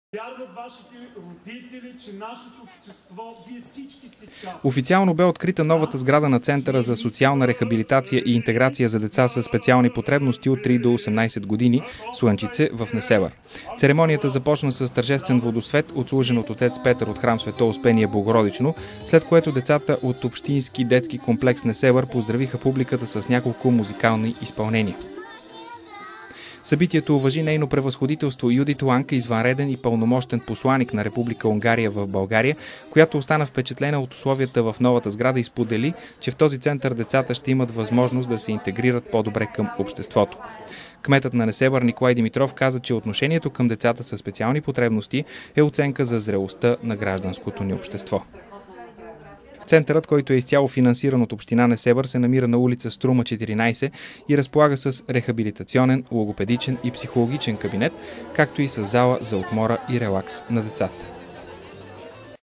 Николай Димитров и посланик на Унгария
Унгарската посланичка и кметът на Несебър откриха новата сграда на Центърът за социална рехабилитация и интеграция за деца със специални потребности.